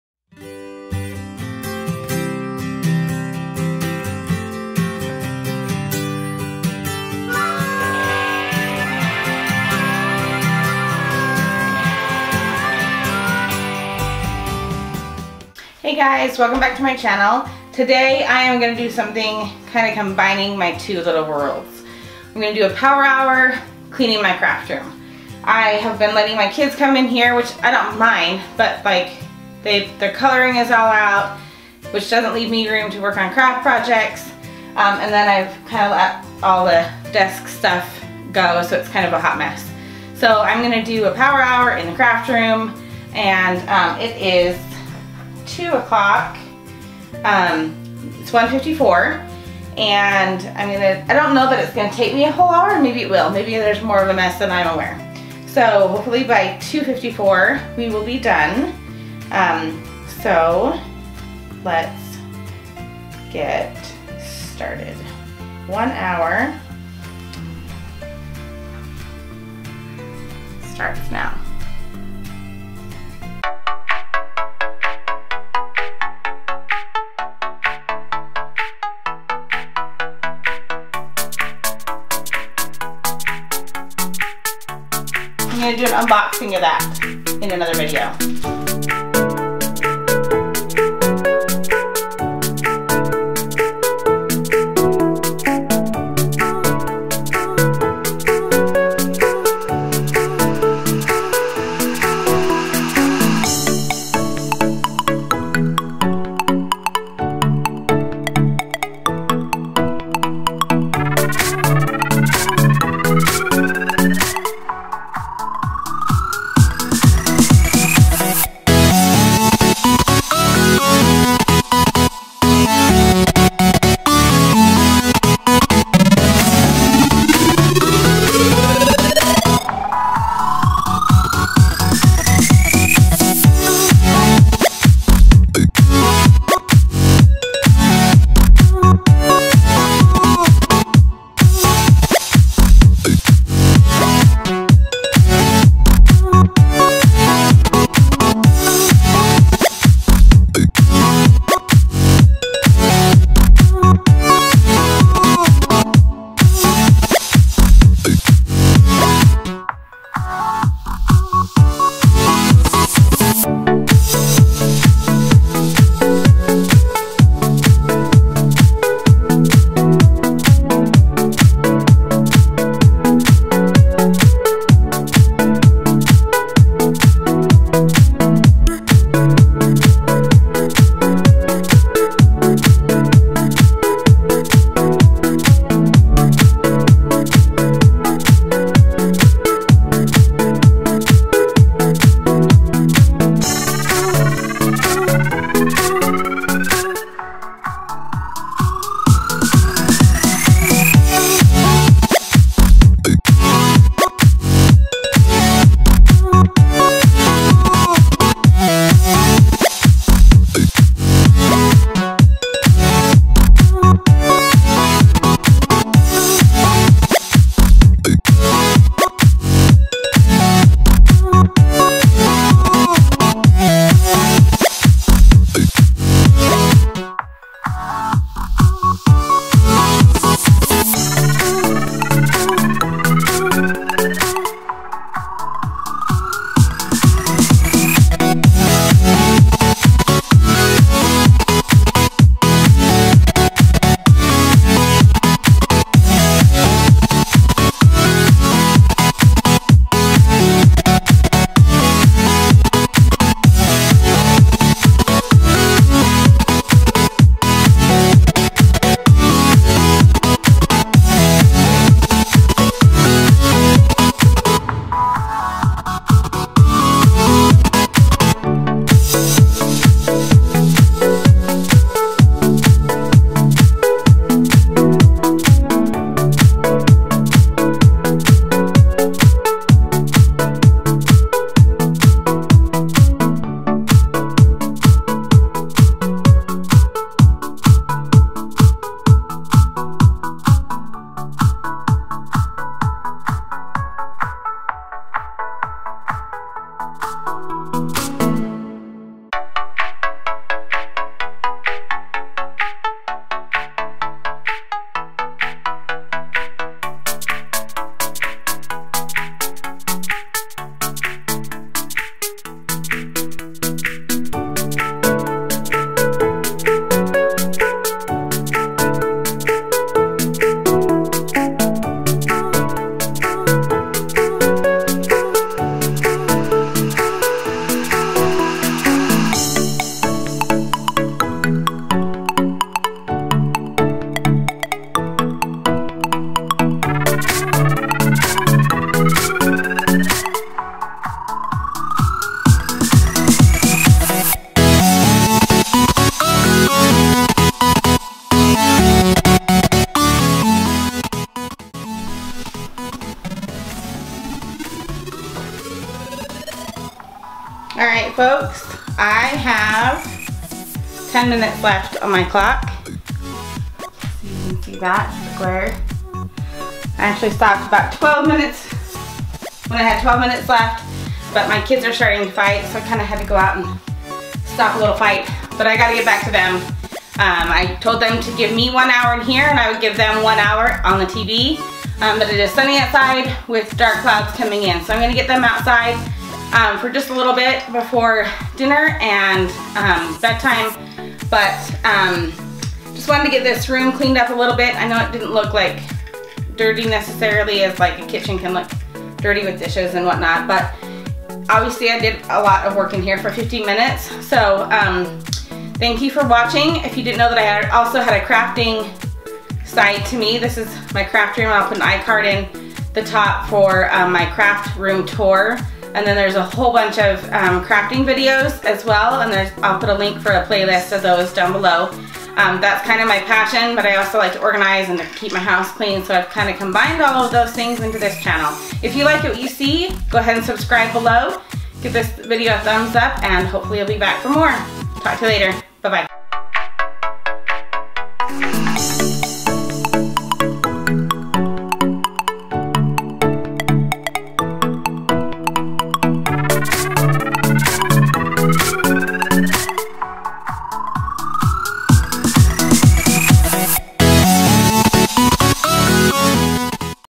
Power Hour #8: Speed Cleaning my Craft Room
Welcome to yet another Power Hour! Today I really needed to get my craft room cleaned up so that I can film some craft videos next week.  I've also done a tour video of my craft room as well. I'll link it below, and it'll be in an i-card in this video. :)